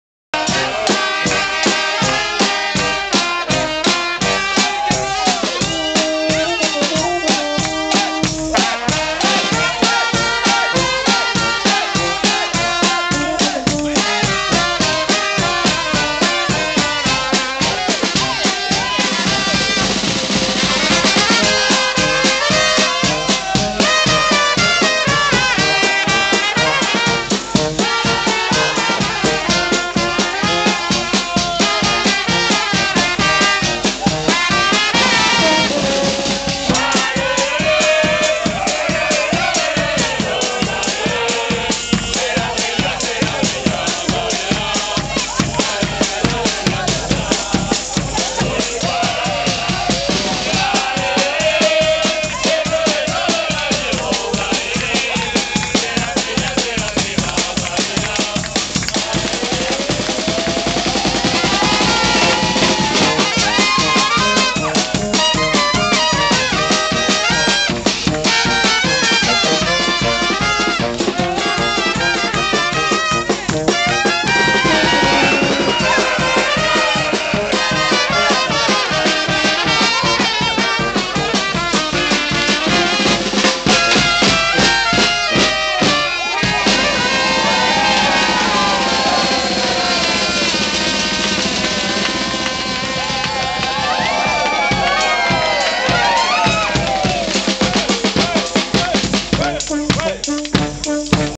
bandazik au moulin de paulhiac 1
Soirée périgourdine  au camping du moulin de Paulhiac à Daglan dans le Périgord noir Aout 2009